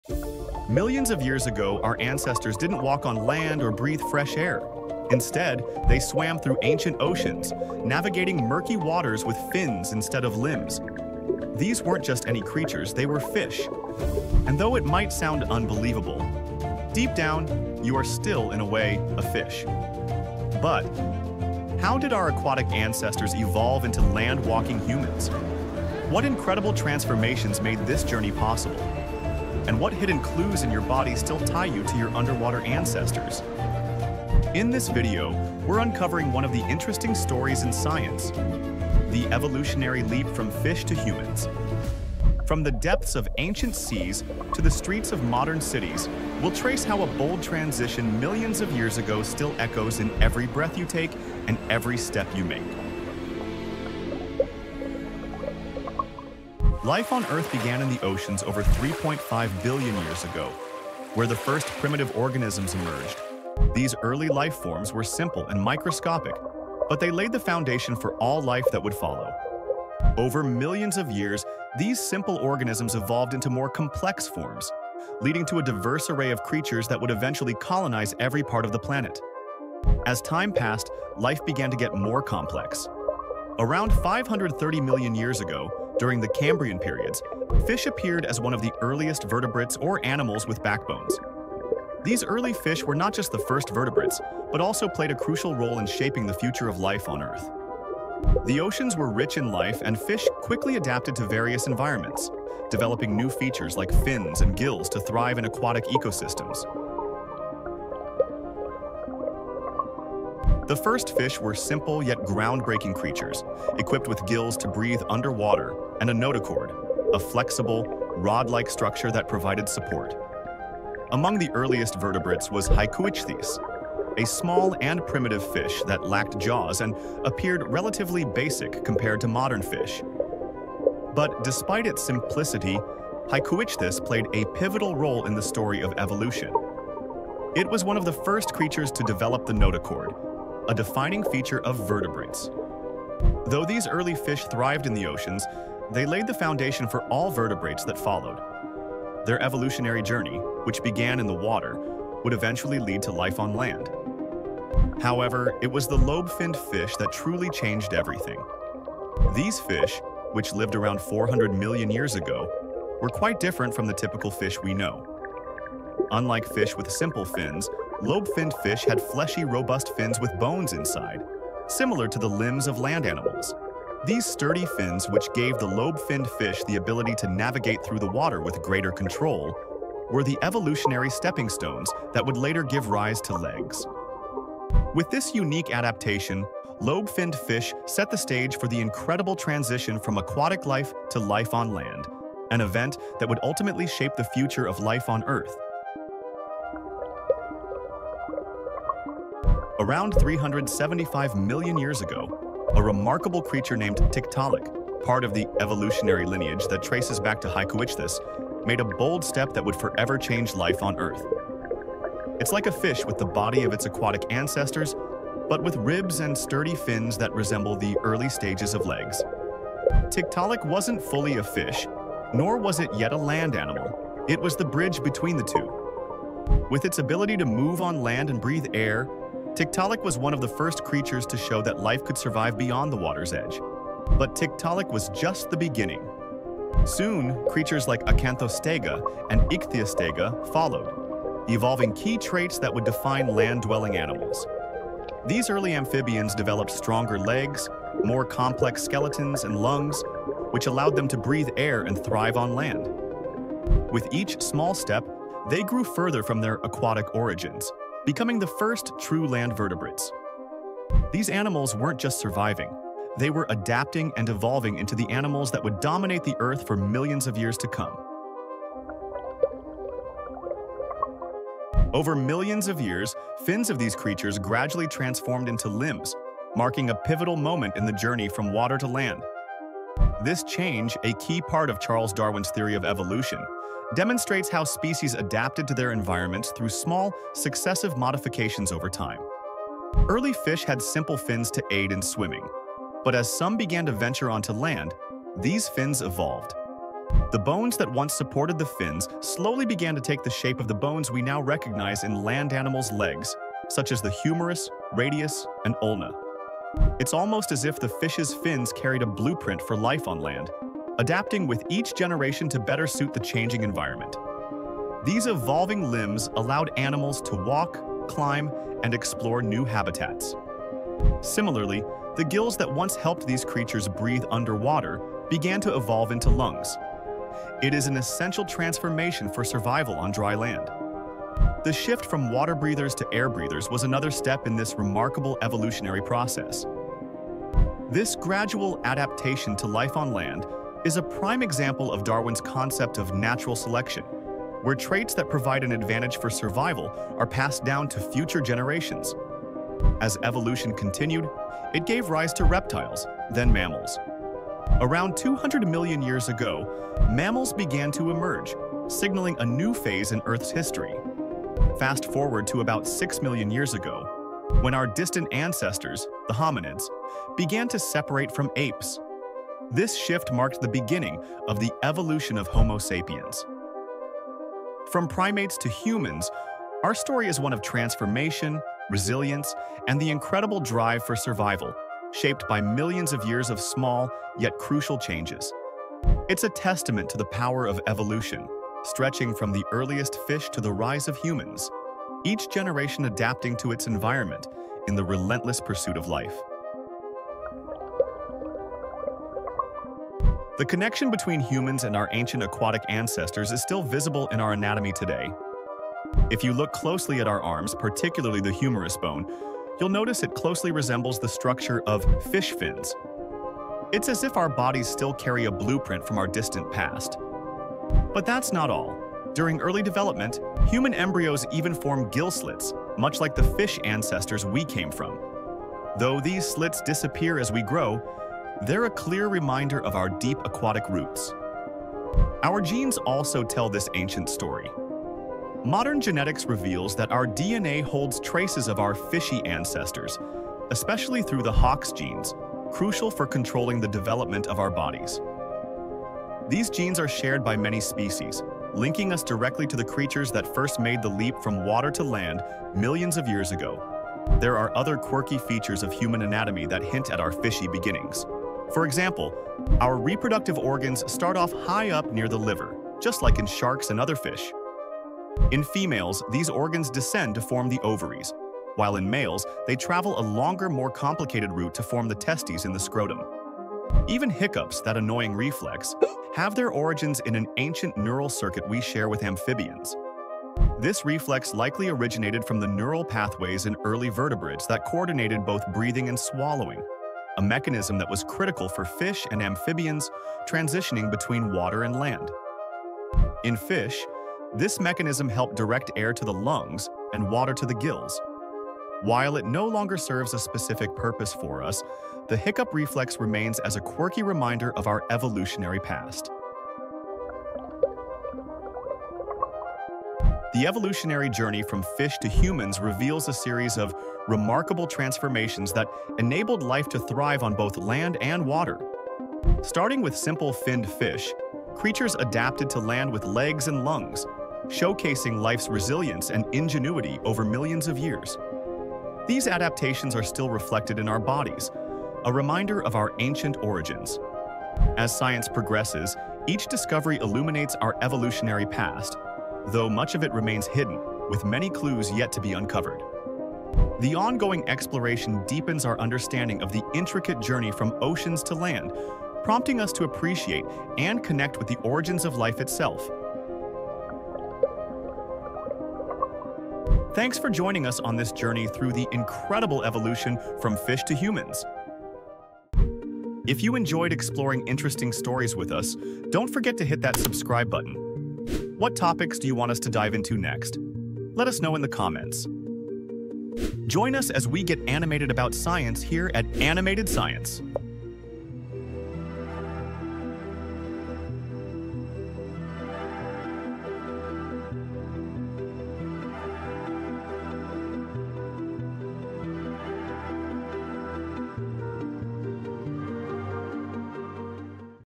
Did Humans Really Evolve from Fish? (The Evolutionary Journey of Homo Sapiens) | Animated Science
In this video, we’ll dive into the origins of life in the oceans and uncover the critical role fish played in the rise of land animals. We’ll trace the fascinating evolutionary journey that led to the first creatures stepping onto land, and meet Tiktaalik, the “fish” with legs, a creature that bridged the gap between aquatic and land life. Learn how the lobe-finned fish paved the way for land-dwelling vertebrates and how evolution slowly transformed fins into limbs and gills into lungs, forever changing life on Earth.

As we delve into genetic evidence and quirky anatomical clues found in humans, you’ll see how our deep connection to fish is still reflected in our bodies today. From the structure of our limbs to even the hiccups we experience, every part of us tells the story of our watery past.

Join us on this animated adventure through time, where we reveal the extraordinary leap from fish to humans, and show how this ancient transformation still shapes who we are today.

Timestamp:
0:00 Did Humans Evolve from Fish
0:56 First life in the Oceans
1:51 The first fish and their importance to evolution of land animals
3:19 How Tiktaalik Bridged the Gap Between Fish and Land Animals?
4:31 How did evolution allow fish to transition to land?
6:39 Genetic and physical evidence of humans evolving from fish